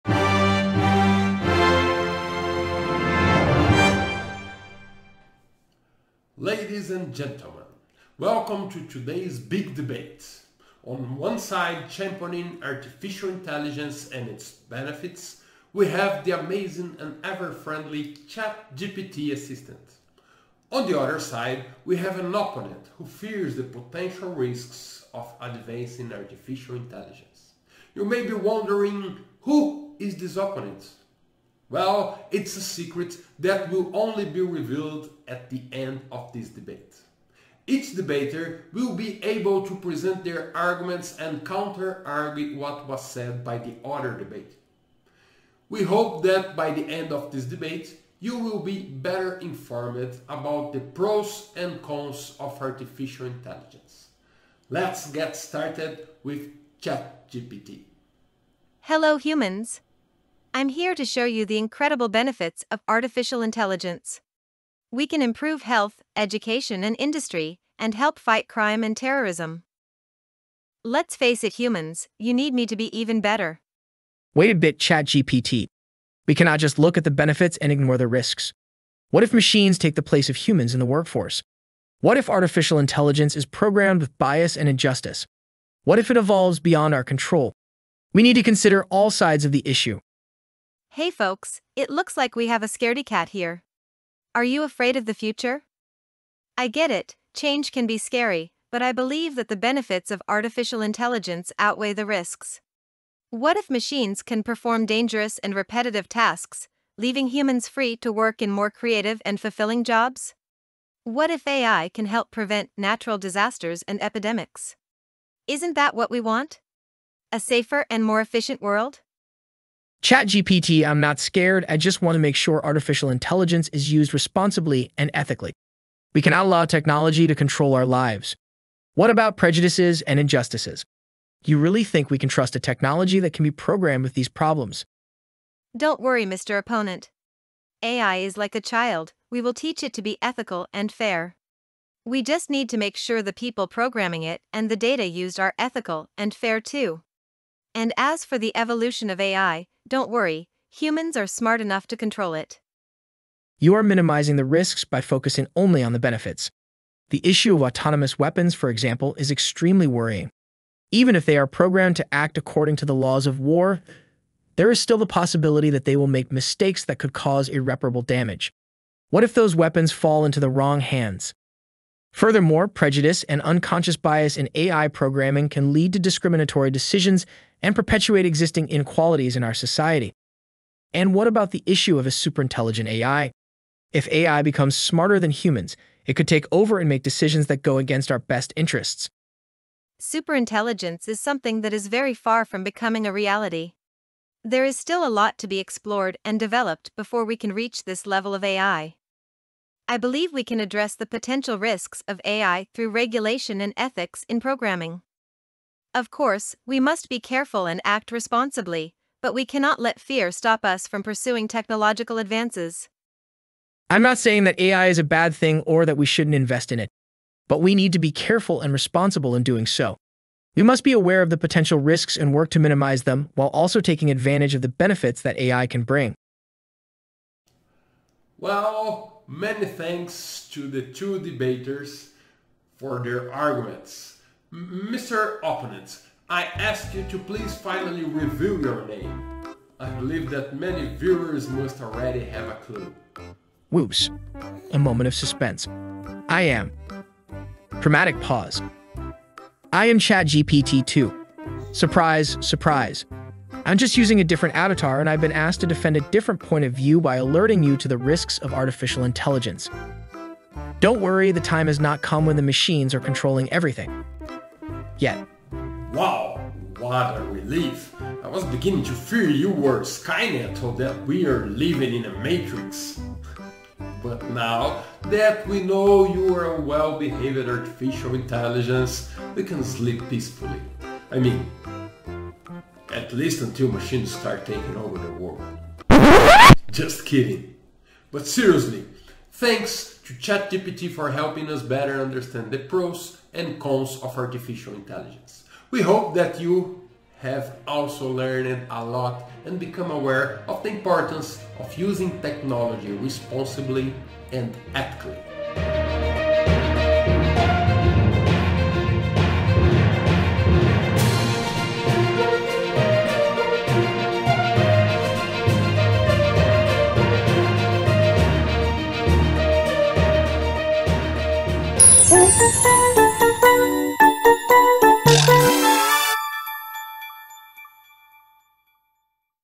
The Big Debate: Risks and Benefits of Artificial Intelligence
ChatGPT is already a regular in The Brazilian BA Channel. Now she faces a worthy opponent to debate the risks and benefits of artificial intelligence. Join us as we discuss the ethical and practical implications of AI and the steps we can take to ensure its responsible and positive use.